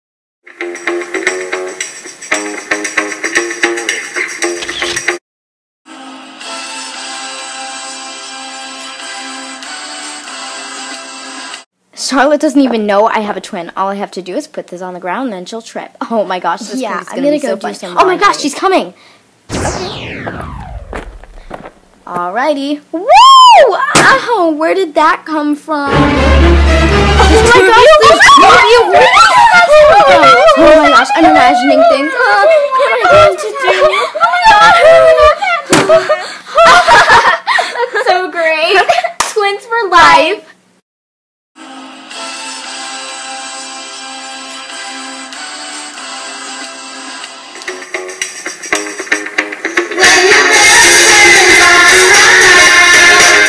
The Halloween prank!
Happy Halloween! I hope you enjoy this video I made with my twin sister, Misty a.k.a. dreamer studios we made it with our friends doll and then I did the voiceovers for it  with my sister. ~Elita